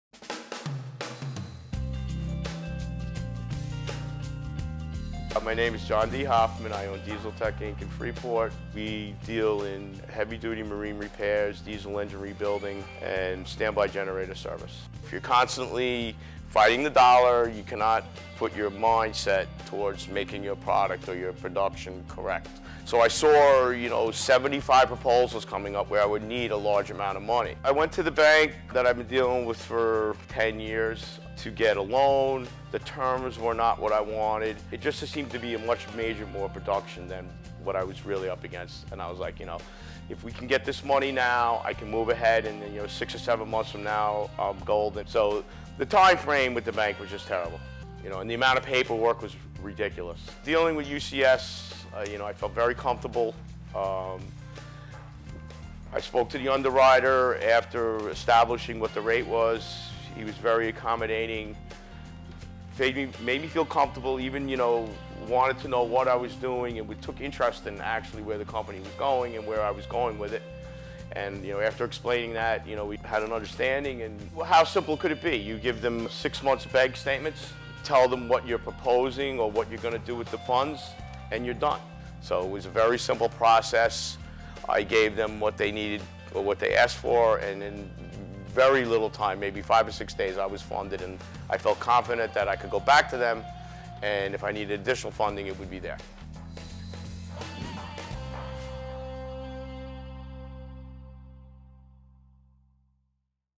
Marine Equipment Video Testimonial for Small Business Funding
View a video of John D. Hoffman, owner of Diesel Tech Inc., who speaks about the short 5 to 6 day approval process for a business loan with United Capital Source.